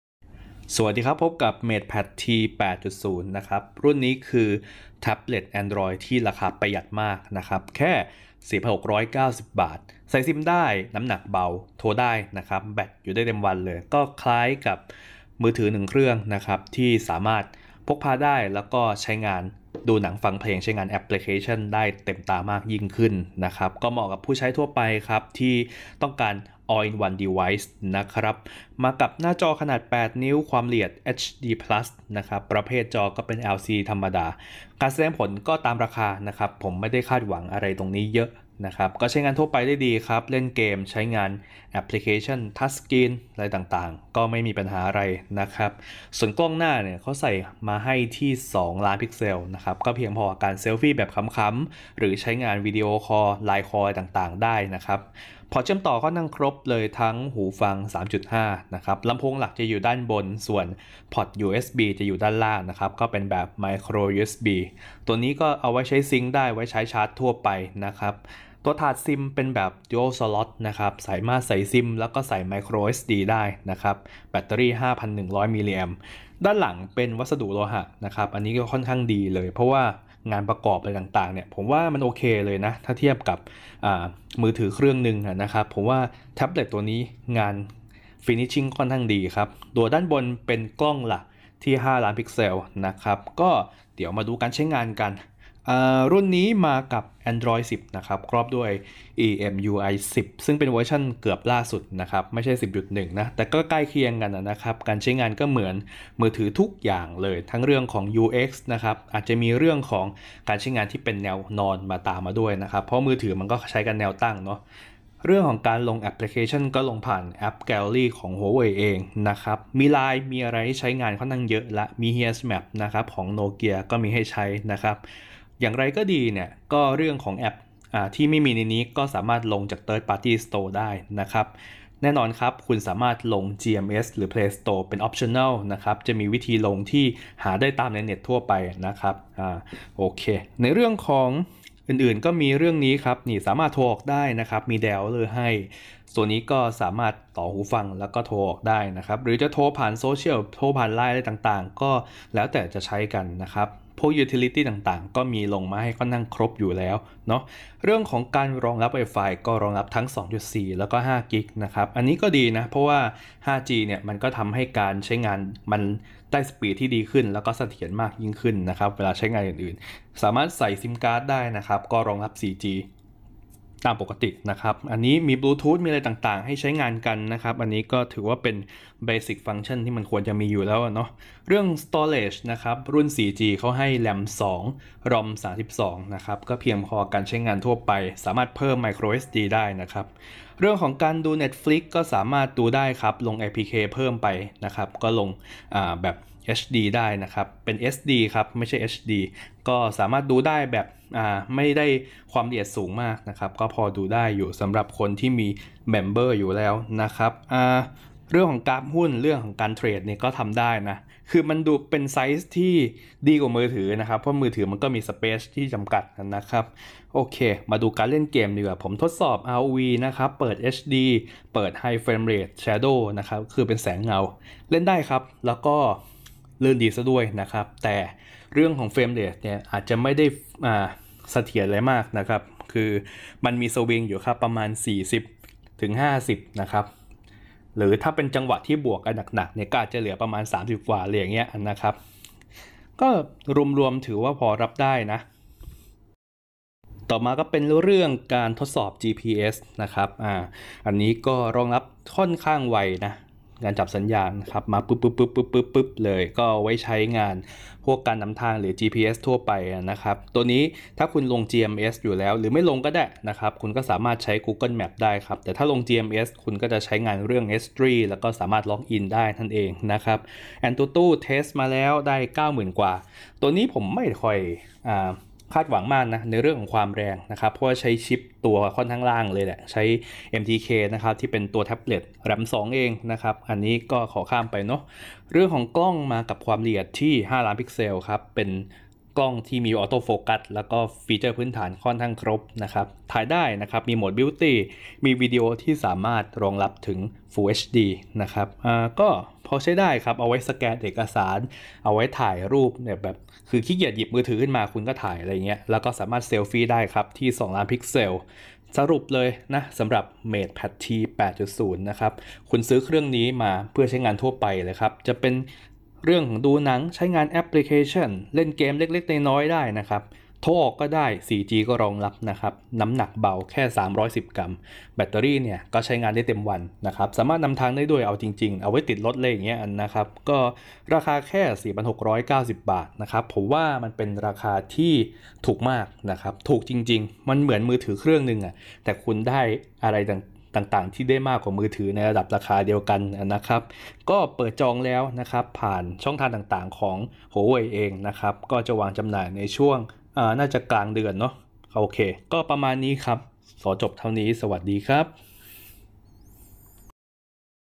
รีวิว Huawei MatePad T 8.0 จอใหญ่ ใส่ซิม 4G แบตอึด น้ำหนักเบา งบ 4,xxx บาท
คนถามเยอะ MatePad T 8.0 แท็บเล็ตราคาประหยัด ครบทุกอย่างในตัว หน้าจอขนาดใหญ่ 8 นิ้ว ขนาดพอดีมือ หนัก 310 กรัม, ชิพ Octa-Core + แรม 2GB, Android 10 EMUI 10 ฟีเจอร์ครบ ลงแอพฯ ผ่าน AppGallery (หรือลง GMS เป็นออฟชั่นเสริม), กล้อง 5 ล้าน / กล้องหน้า 2 ล้าน แบต 5100 mAh พร้อมใส่ซิม 4G โทรออกได้นะจ๊ะ

Huawei จัดมาครบ ไม่ต้องติดเยอะ MatePadT8 ขายราคา 4,690 บาท (2GB+32) วาปด้านล่างเลย

(Full Review) MatePad T 8.0 Affordable Android Tablet. Big Screen 8.0" (HD+) but Portable, Light Weight (310 g.), Android 10 + EMUI 10 Full Functionality with 4G + WiFi. Look Like Huge Smartphone . I Recommended 4G LTE Model